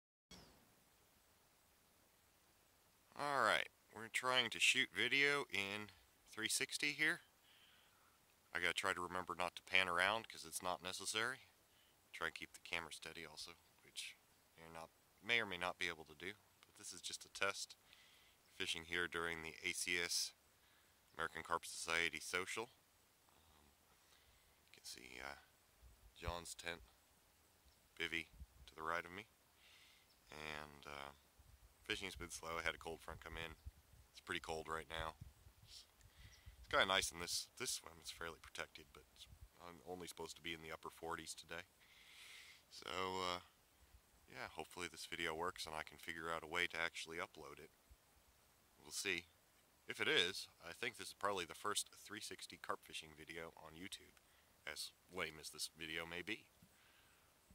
360 Degree Carp fishing ***test***
I did a quick search of 360 degree carp fishing videos on YouTube and did not get anything. So, here is the first one. This is just a test of the idea - I will put up a full video soon! Let me know if this is something you would like to see!